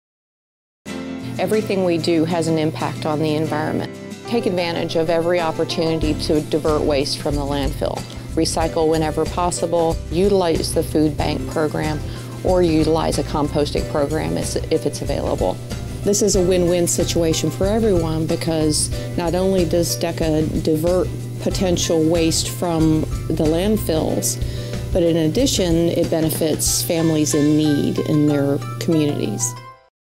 Your Commissary: The Environment and Community
Recycling, composting and food bank cooperation. There are many ways you and Your Commissary come together to support the environment, year round!